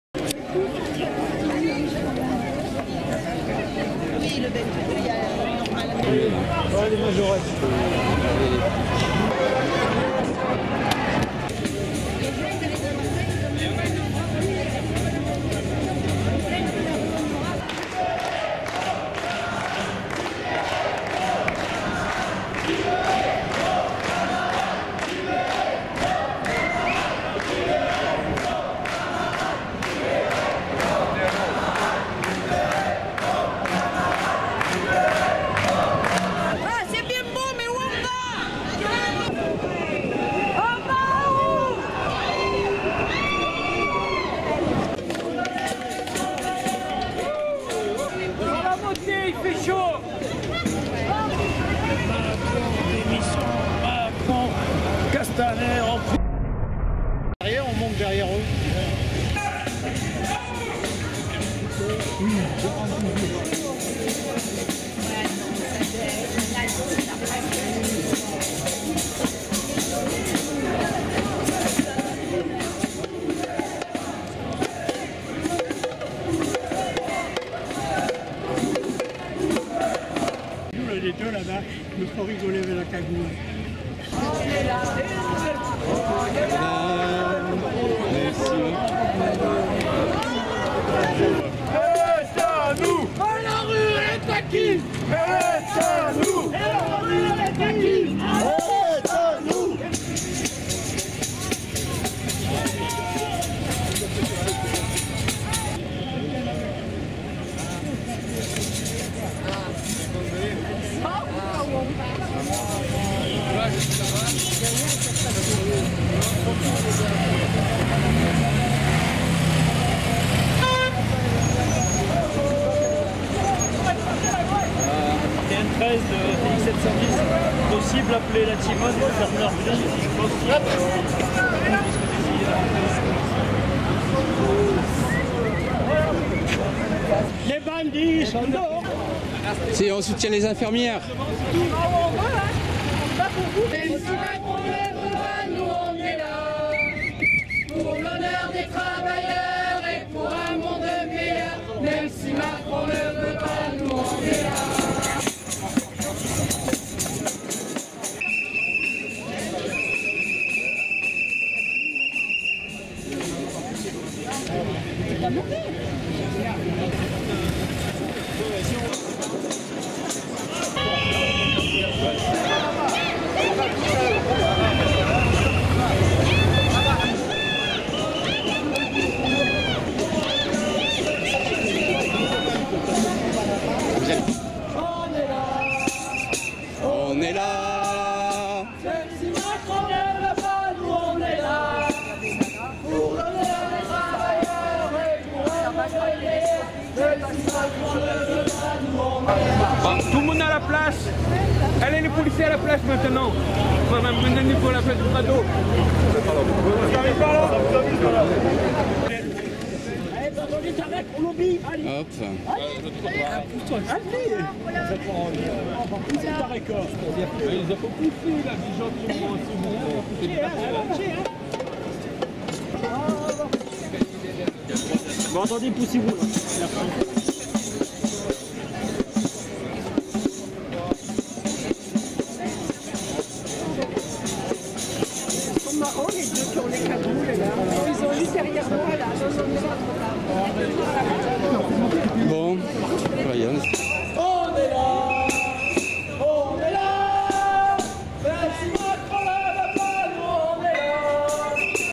Acte 30 Marseille Gilets jaunes Mon résumé
Commissariat de Noailles où des femmes de chambre de l'hôtel NH Collection sont retenues, la Timone en soutien aux grèves des urgences, mobilisation particulièrement faible ce jour là à Marseille, l'occasion de faire le point sur le mouvement

À samedi, 14h, Vieux-Port